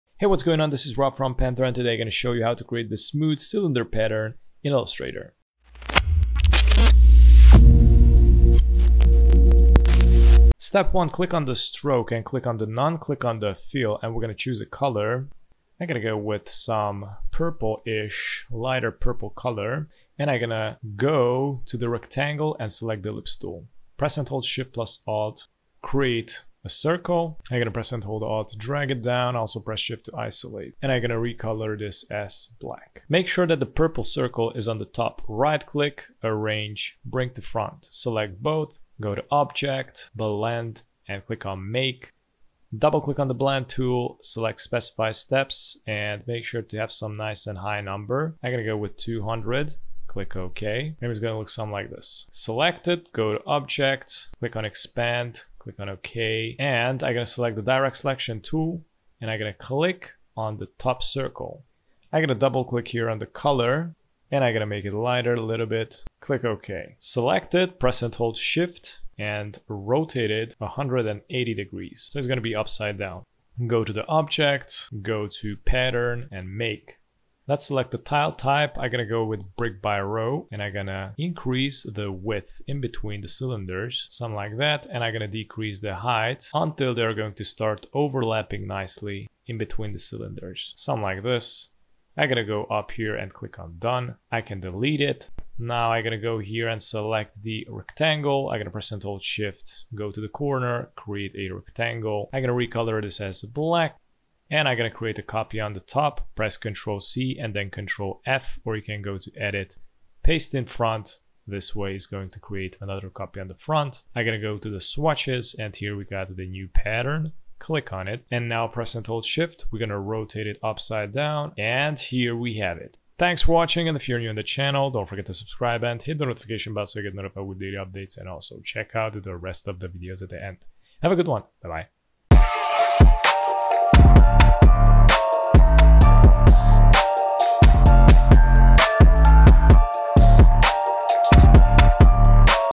How to Create Blend Pattern in Illustrator
The Blend tool in Adobe Illustrator is a feature that allows users to create smooth transitions between two or more objects. By using the Blend tool, users can easily create blends, gradients, and color variations between objects. The tool can be used to create a wide range of effects, such as blending colors, shapes, and text. The resulting blend is a set of objects that smoothly transition from one object to the next, creating a seamless and natural look.

0:00 Intro
0:10 Color setup
0:20 Create cylinder
1:12 Create pattern
2:12 Outro